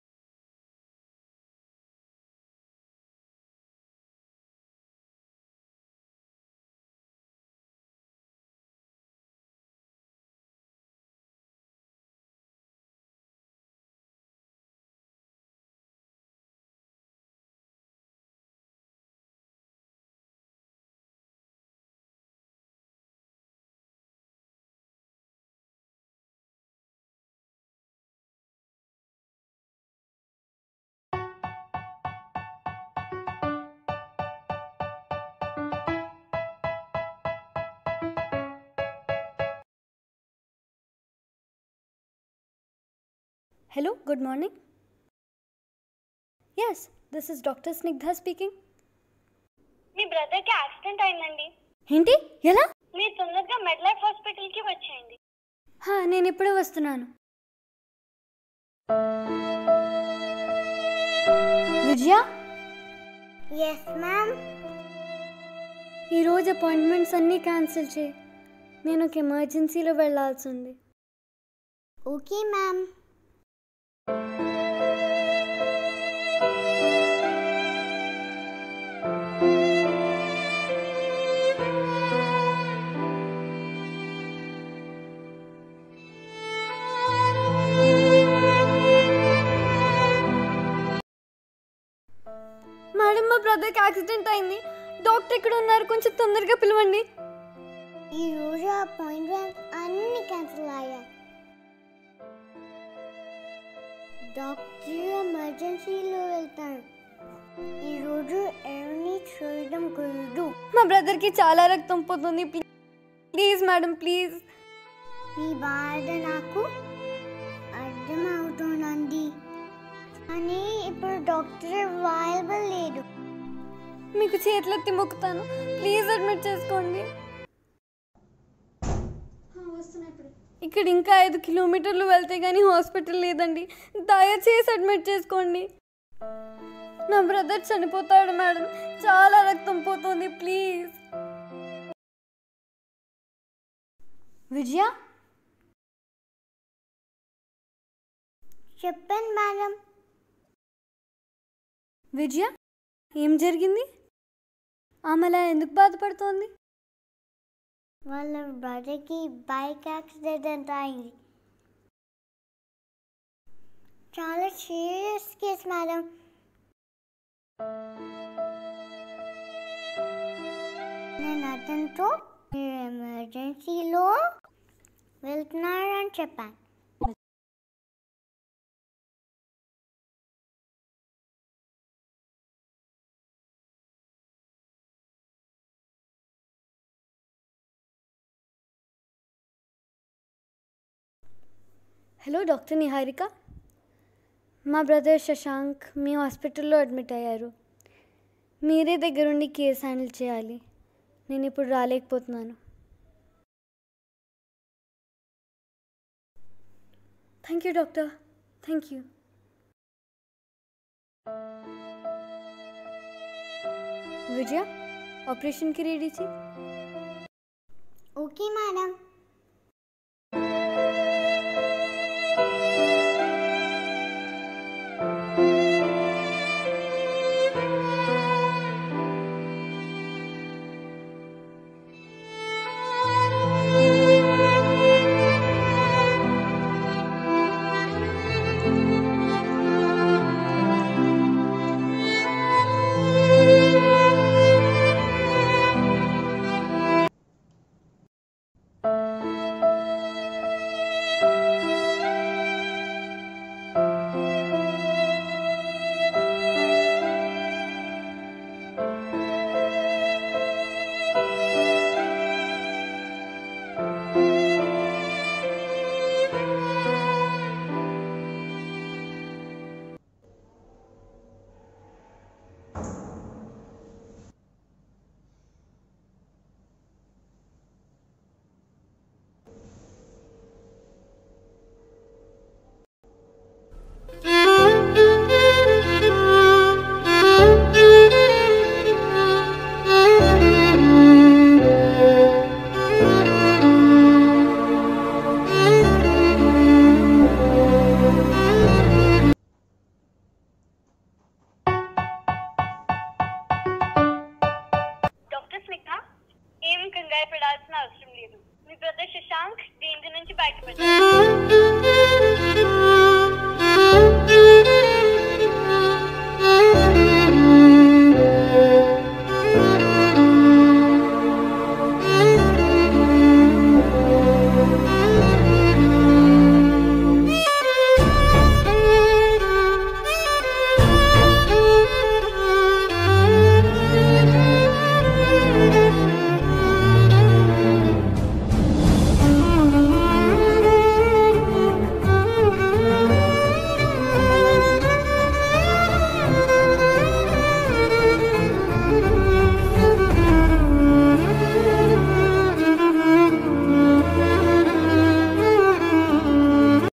||Happy Doctor's Day ||SoldierInWhite || A tribute to Doctors by the lovely duo
Tribute to doctors.. A short film by two lovely sisters